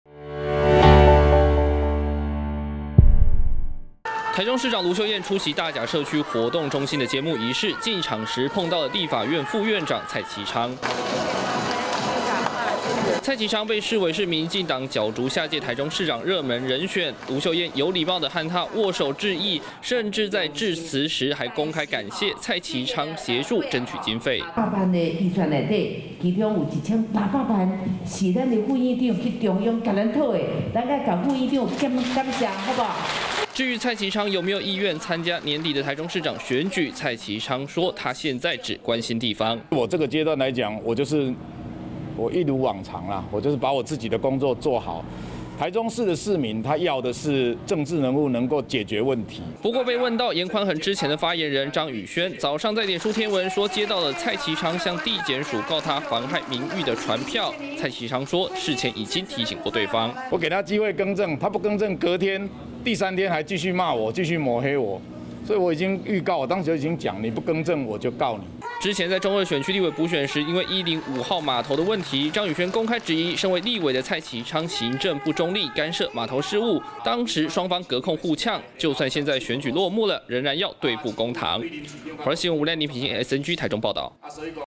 不見戰中市煙硝味! 盧蔡同台"握手致意"｜華視新聞 20220113
中二選區立委補選結束後，接下來就是年底的地方縣市首長選舉，而被視為會代表民進黨，角逐下一屆台中市長的立法院副院長蔡其昌，上午和現任台中市長盧秀燕，一起參加大甲社區活動中心揭幕，雙方見面有簡單握手致意，盧秀燕還公開感謝蔡其昌爭取中央經費，而蔡其昌受訪時只說，現在只想把地方的事情做好。